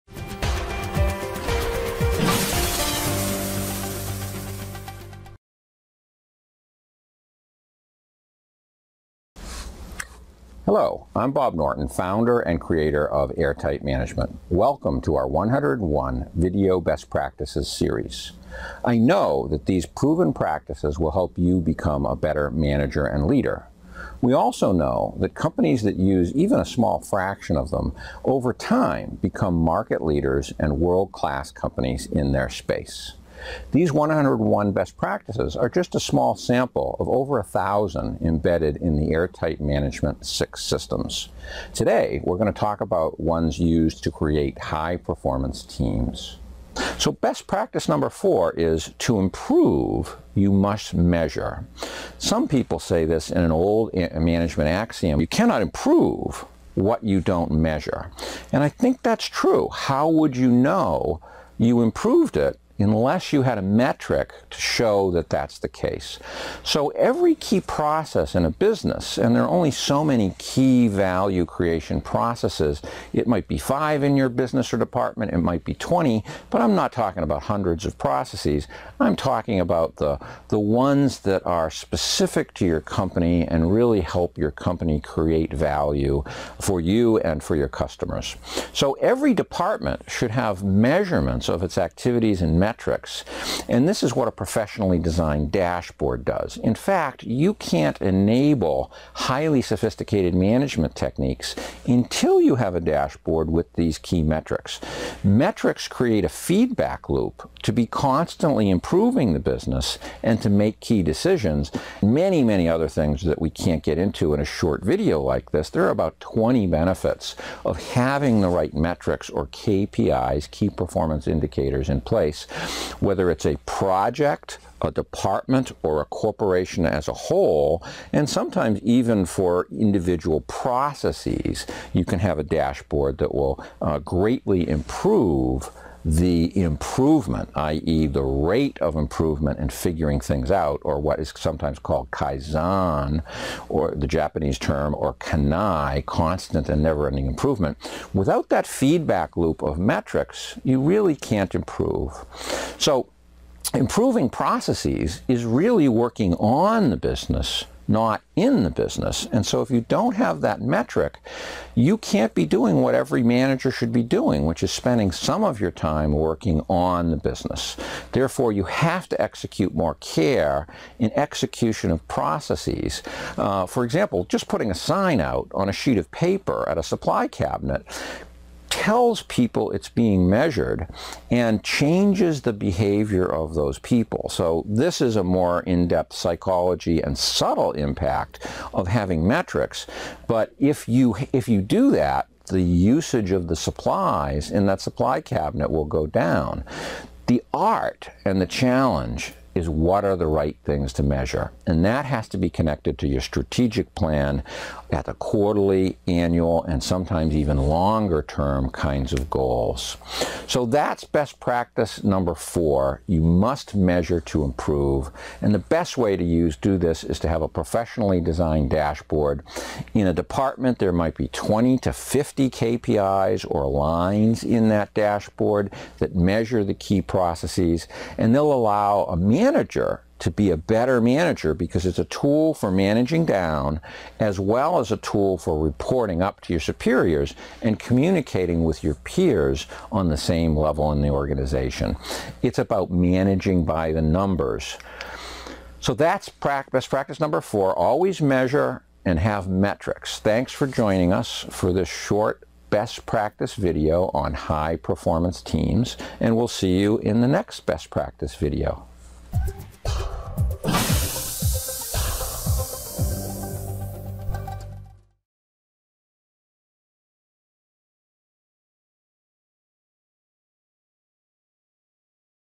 Best Practices Series High-Performance Teams #4 - To Improve You Must Measure
Peter Drucker, The Father of Management said "You cannot improve what you do not measure". Of course, this is literally true, how would you know?
Quantitative measure creates the ability for feedback loops, Kaizen and constant improvement and bakes it into a culture and management cadence at each level. This can be real-time, daily, weekly and monthly at various levels of the organization and guarantees constant improvement becomes cultural.